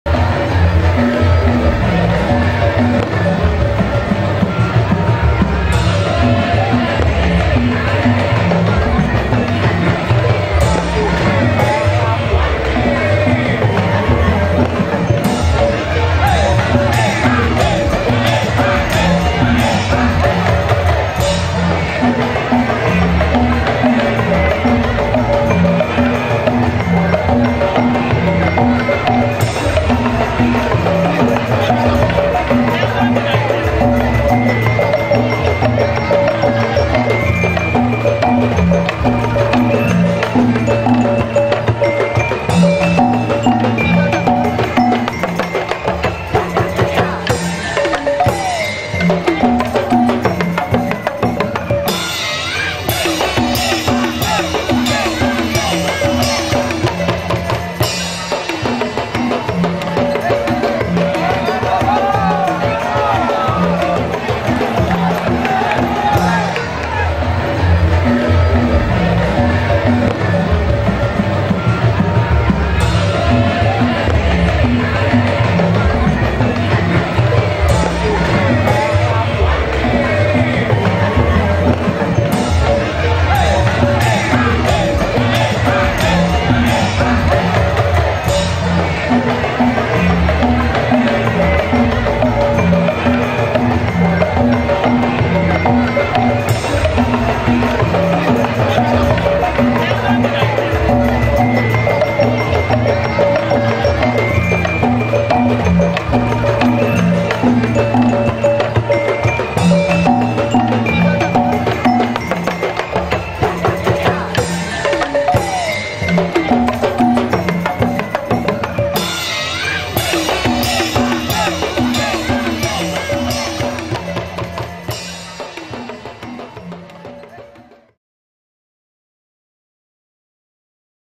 Karnaval Jeruk Lakarsantri 2017 - Part. 17
Karnaval, Kirab, Arak Arakan, Acara sedekah bumi, acara tegal deso,  warga jeruk lakarsantri Surabaya.